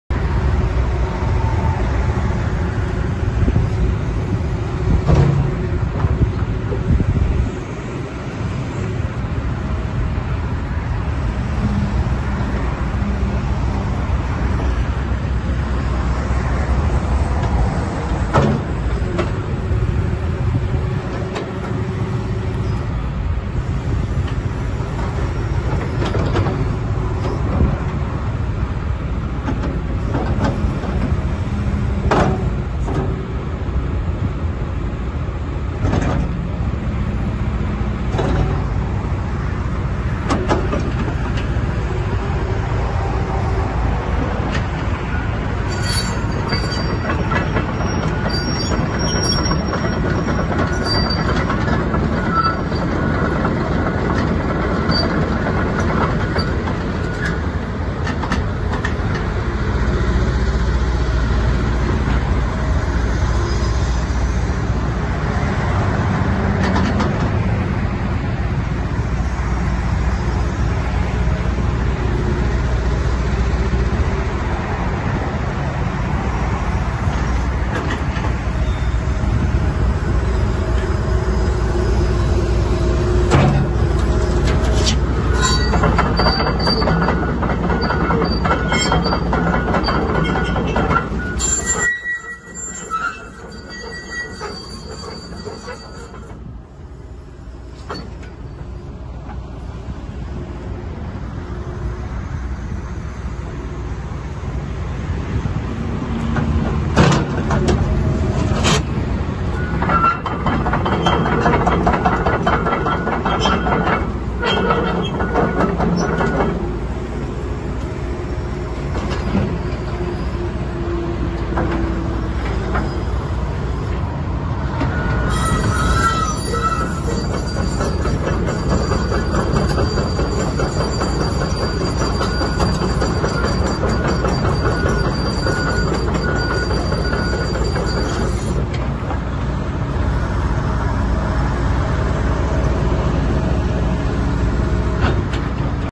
2012 DEERE 225D LC For Sale
Isuzu; 4HK1-XDIAA-01-C2; 159 HP; EPA STICKER VISIBLE; Bkt Make & Size: Johh Deere 42"; Hyd Q/C; Hyd Thumb; Aux Hyd; A/C Cab; Zero Tail Swing; Pad Size & Type: 32"